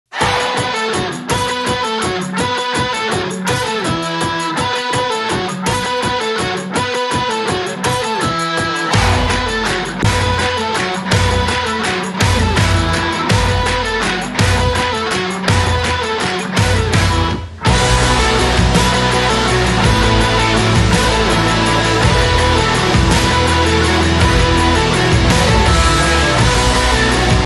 Painting Scottie Barnes with a basketball as a paintbrush
Scott Wayne Barnes Jr. is an American professional basketball player for the Toronto Raptors of the National Basketball Association.